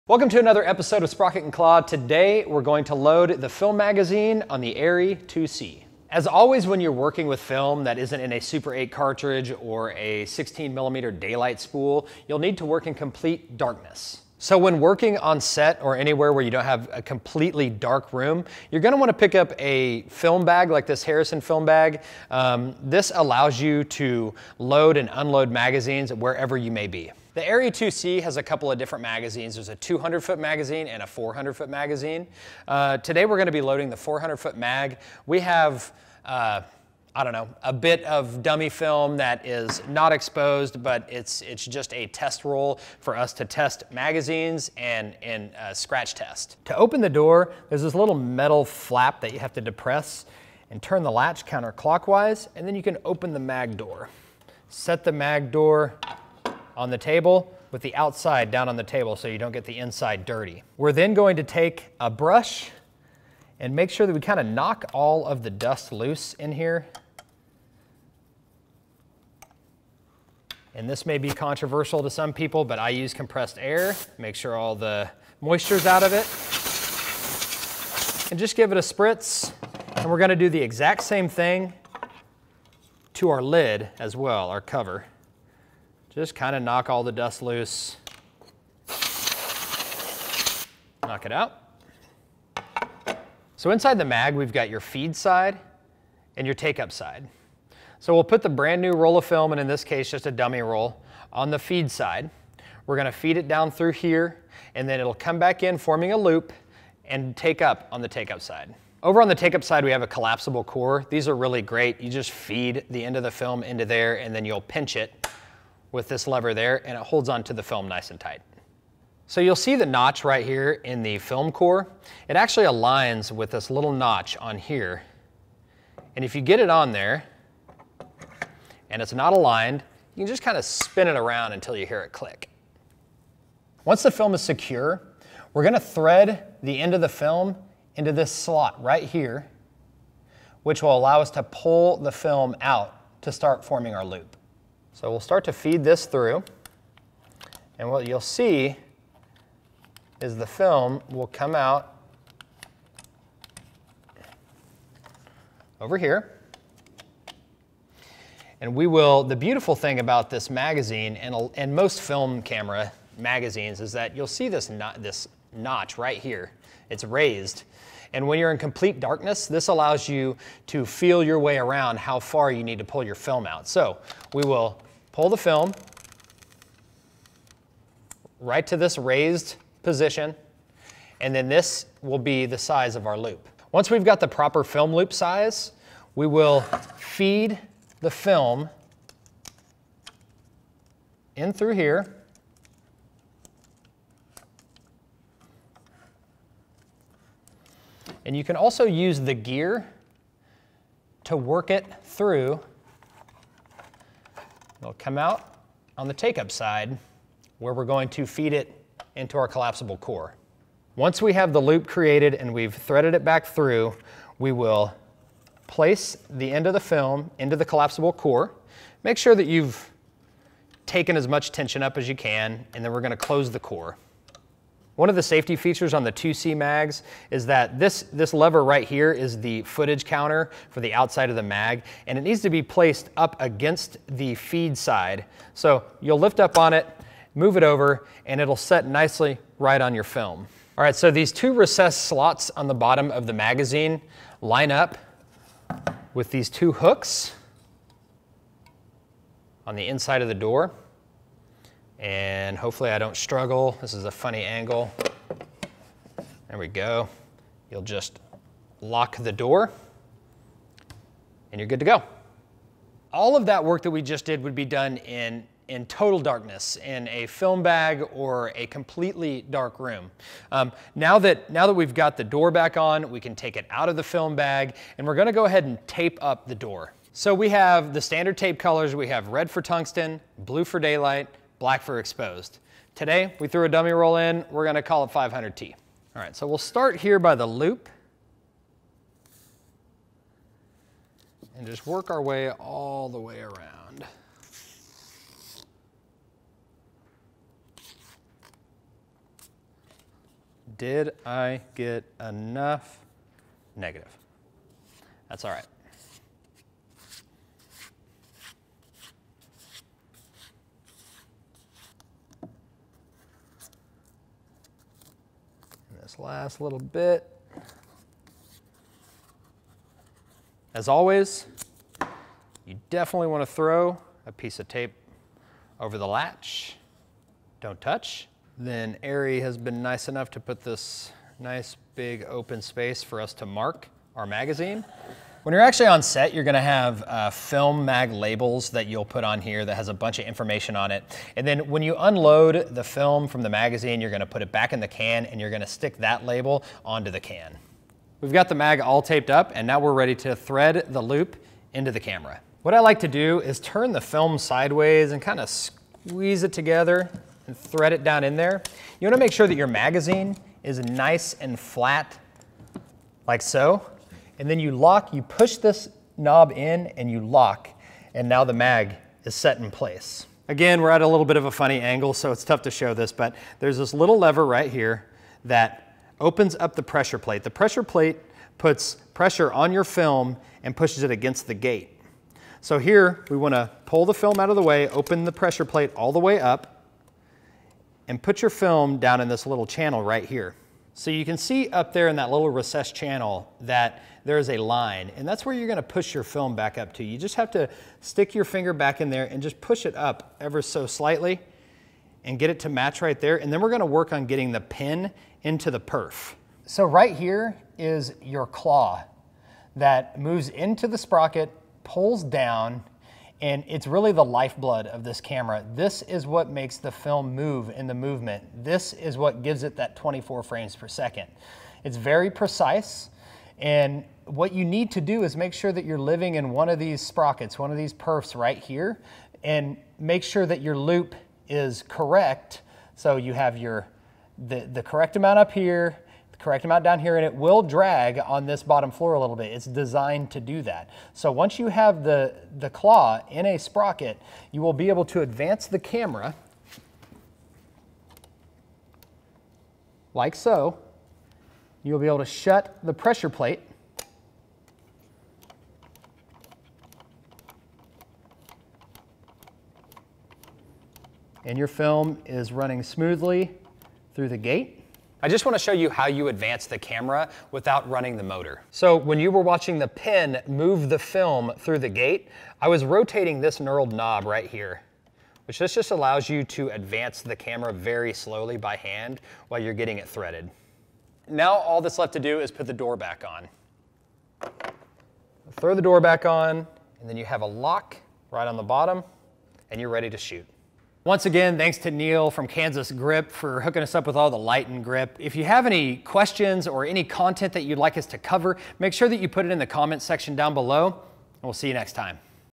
How to Load an Arri 2C Film Magazine - Sprocket&Claw - Ep 006
In this episode we cover how to load the film magazine for the ARRI 2C 35mm film camera. Little techy... but we needed content, so hey, hope you learn something today! Also, Stanley Kubrick operated a 2C... a lot, so there's that.

Camera: Arriflex 2C